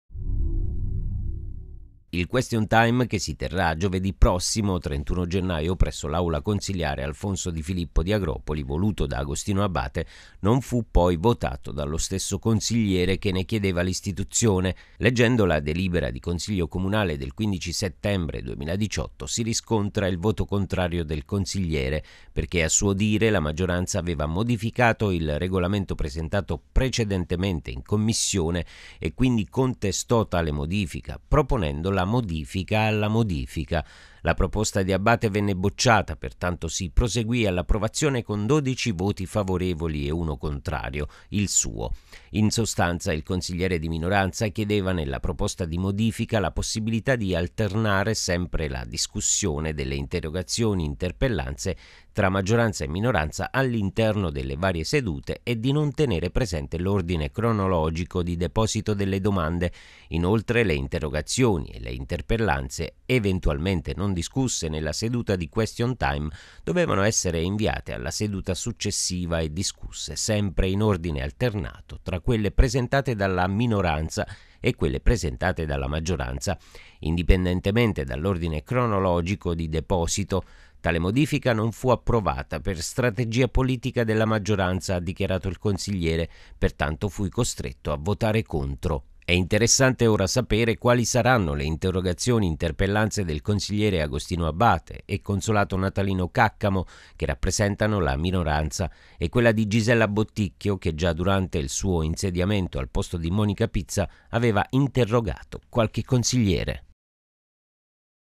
AGROPOLI, MOTIVAZIONI DEL CONSIGLIERE ABATE SUL VOTO CONTRARIO ALL'APPROVAZIONE DEL "QUESTION TIME".
AGROPOLI, LE MOTIVAZIONI DEL CONSIGLIERE ABATE SUL VOTO CONTRARIO ALL'APPROVAZIONE DEL "QUESTION TIME".